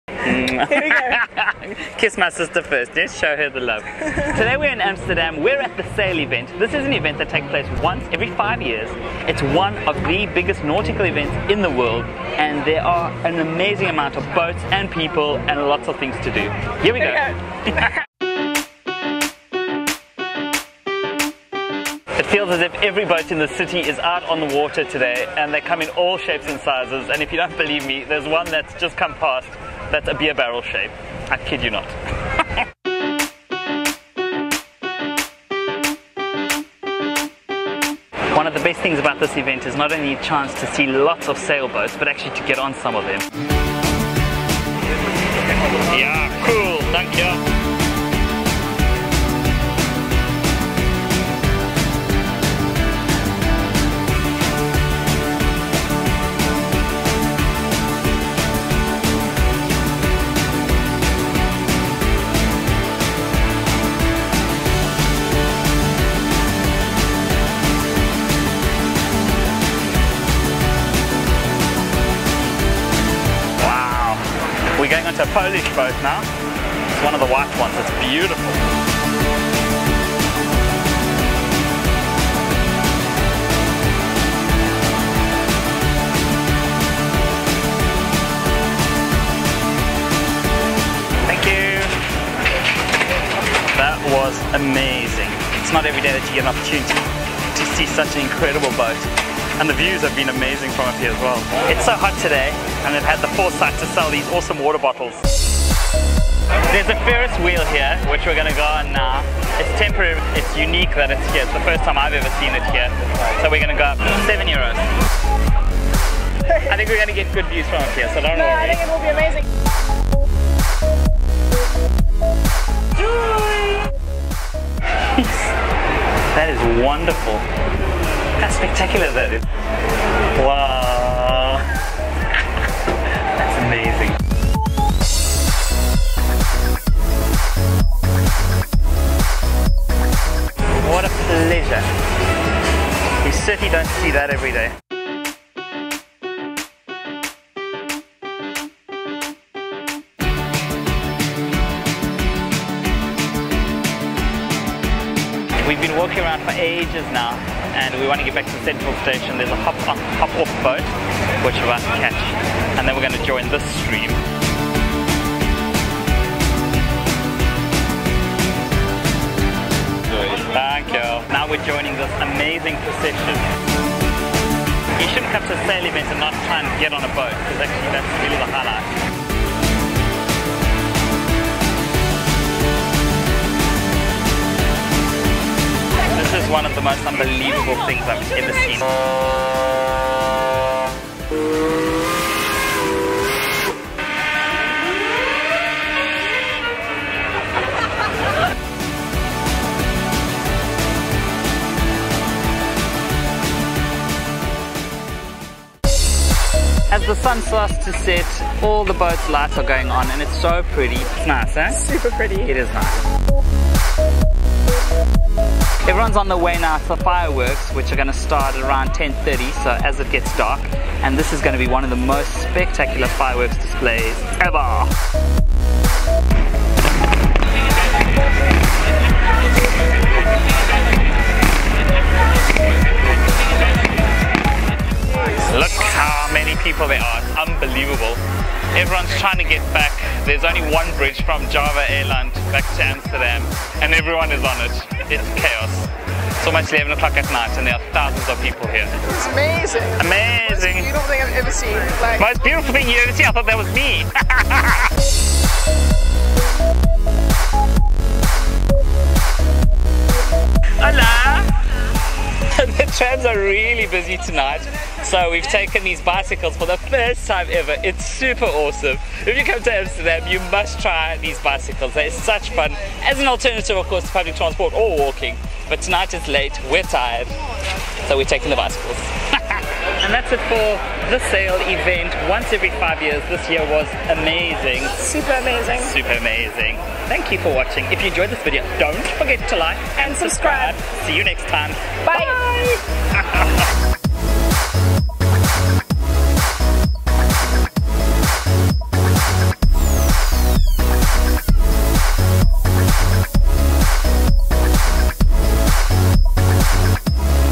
Amsterdam Sail 2015
The Sail Amsterdam event takes place every five years. It's one of the biggest nautical events in the world. 2015 was the ninth edition where 44 tall sail ships from all over the world participated. There was an amazing number of boats, people and activities around Amsterdam North.

Music and sound effects are provided by Jinglepunks.com.